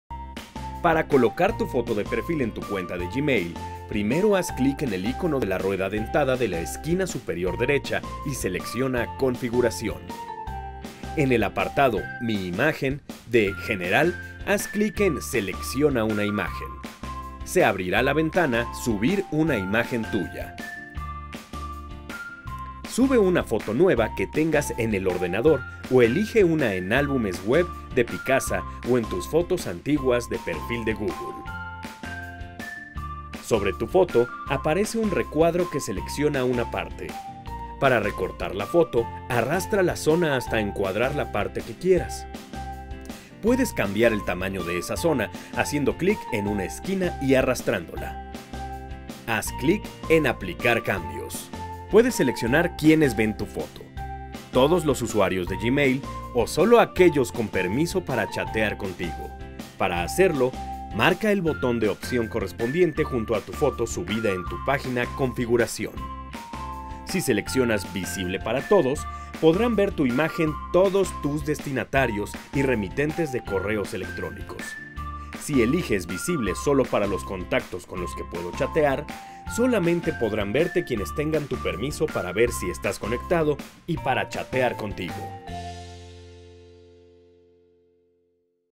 ¿Cómo seleccionar tu foto de perfil en Gmail? | UTEL Universidad
En este Tutorial aprenderás a personalizar tu foto de perfil en Gmail.

¡Interactúa con el hashtag EstudioEnLínea!